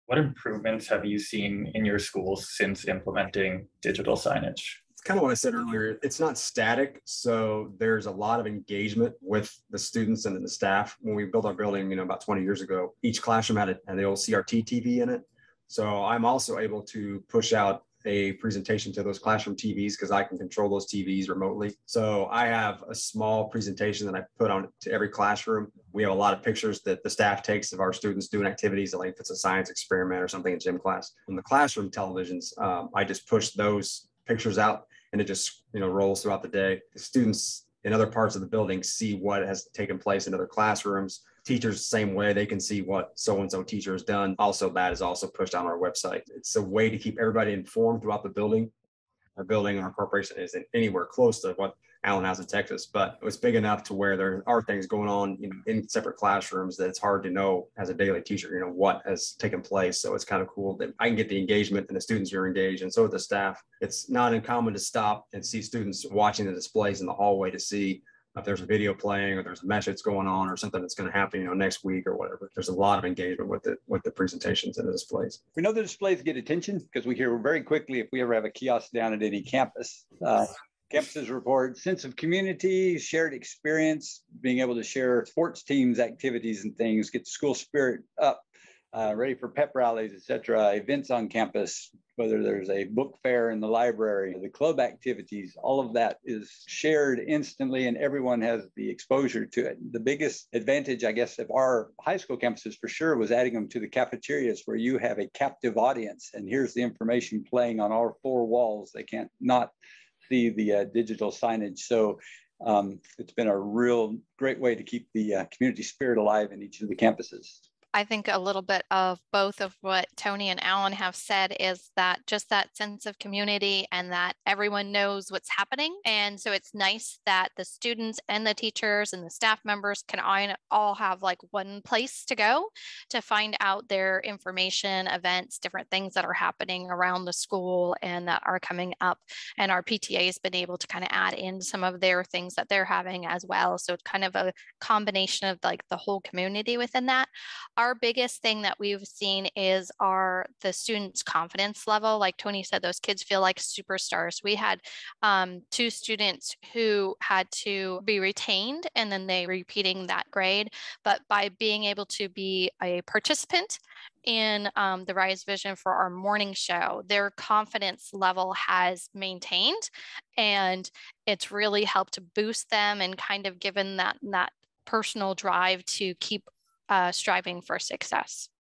What Improvements Have You Seen With Rise Vision?
What improvements have you seen in your schools
since implementing digital signage?

 [0:00 - 1:13] - Tony Smodilla
It's kind of what I said earlier, it's not static, so there's
a lot of engagement with the students and the staff

When we built our building about 20 years ago,
each classroom had an old CRT TV in it

So, I'm also able to push out a presentation to those classroom
TVs because I can control those TVs remotely

So, I have a small presentation that
I put on to every classroom

We have a lot of pictures that the staff
takes of our students doing activities

Like it's a science experiment or something in gym class

In the classroom televisions, I just push those
pictures and it just rolls throughout the day

The students in other parts of the building see
what has taken place in other classrooms

Teachers same way, they can see what
so-and-so teacher has done

That is also pushed on our website, it's a way to
keep everybody informed throughout the building

Our building, our corporation isn't anywhere
close to what Allan has in texas, but...

It's big enough to where there are things going on in separate
classrooms, that's hard to know as a daily teacher

You know, what has taken place, so it's kind
of cool that I can get the engagement

And the students are engaged and so are the staff

It's not uncommon to stop and see students
watching the displays in the hallway to see

If there's a video playing or there's a message going on or
something is going to happen next week or whatever

There's a lot of engagement with it, with the
presentations of this place

 [1:14 - 1:57] - Allan Horton
We know the displays get attention because we hear very
quickly if we ever have a kiosk down at any campus

Campuses report sense of community, shared experience,
being able to share sports teams activities

And things, gets the school spirit up

Ready for pep rallies, et cetera, events on campus

Whether there's a book fair in the library

The club activities, all of that is shared instantly
and everyone has the exposure to it

The biggest advantage, I guess, of our high school campuses
for sure was adding them to the cafeterias

Where you have a captive audience and here's
the information playing on our four walls

They can't not see the digital signage, so...

It's been a real, great way to keep the community
spirit alive in each of the campuses

 [1:58 - 2:59] Joyanne Herdman
I think a little bit of both of what Tony and Allan
have said is that, just that sense of community

And that everyone knows what's happening

And so, it's nice that the students and the
teachers and the staff [and I] all have...

Like one place to go to find out their information, events,
different things that are happening around the school

And that are coming up and our PTA has been able to kind
of add in some of their things that they're having as well

So it's kind of a combination of the
whole community within that

Our biggest thing that we've seen is our students
confidence level, like Tony said...

Those kids feel like superstars

We had two students who had to be retained
and then they [repeated] that grade

But by being able to be a participant in
the Rise Vision for our morning show

Their confidence level has maintained and it's really
helped boost them and kind of given that...

That personal drive to keep striving for success